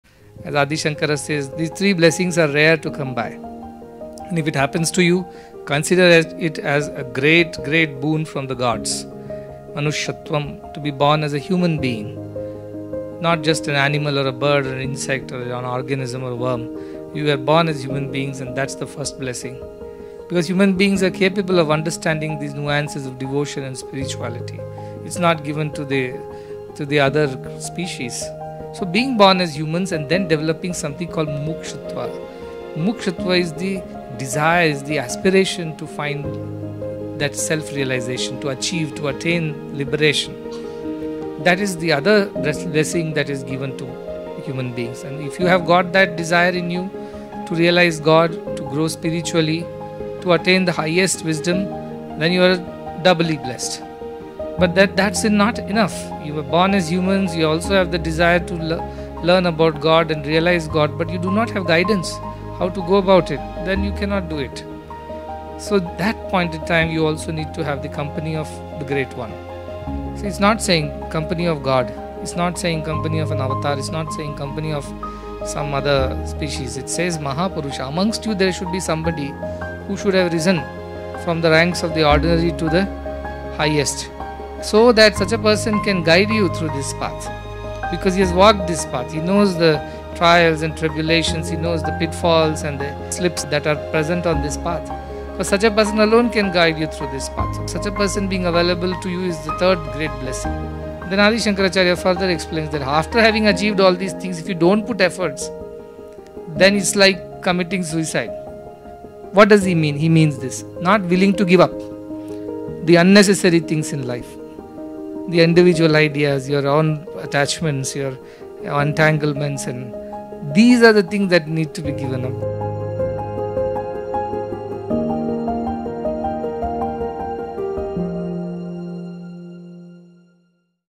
The 3 Rarest Blessings | Sri Madhusudan Sai
In this thought-provoking video, Sri Madhusudan Sai highlights the rarity and profound significance of possessing all three of these blessings simultaneously, echoing the teachings of the revered philosopher Adi Shankara. These blessings are essential for spiritual growth and personal enrichment. Watch now to uncover what they are and how they can empower you on your spiritual journey.

Excerpt from discourse given in Singapore on the 22nd of June, 2024

Official Social Profiles of Sri Madhusudan Sai Global Humanitarian Mission-

Official websites of the Healthcare, Nutrition and Education missions -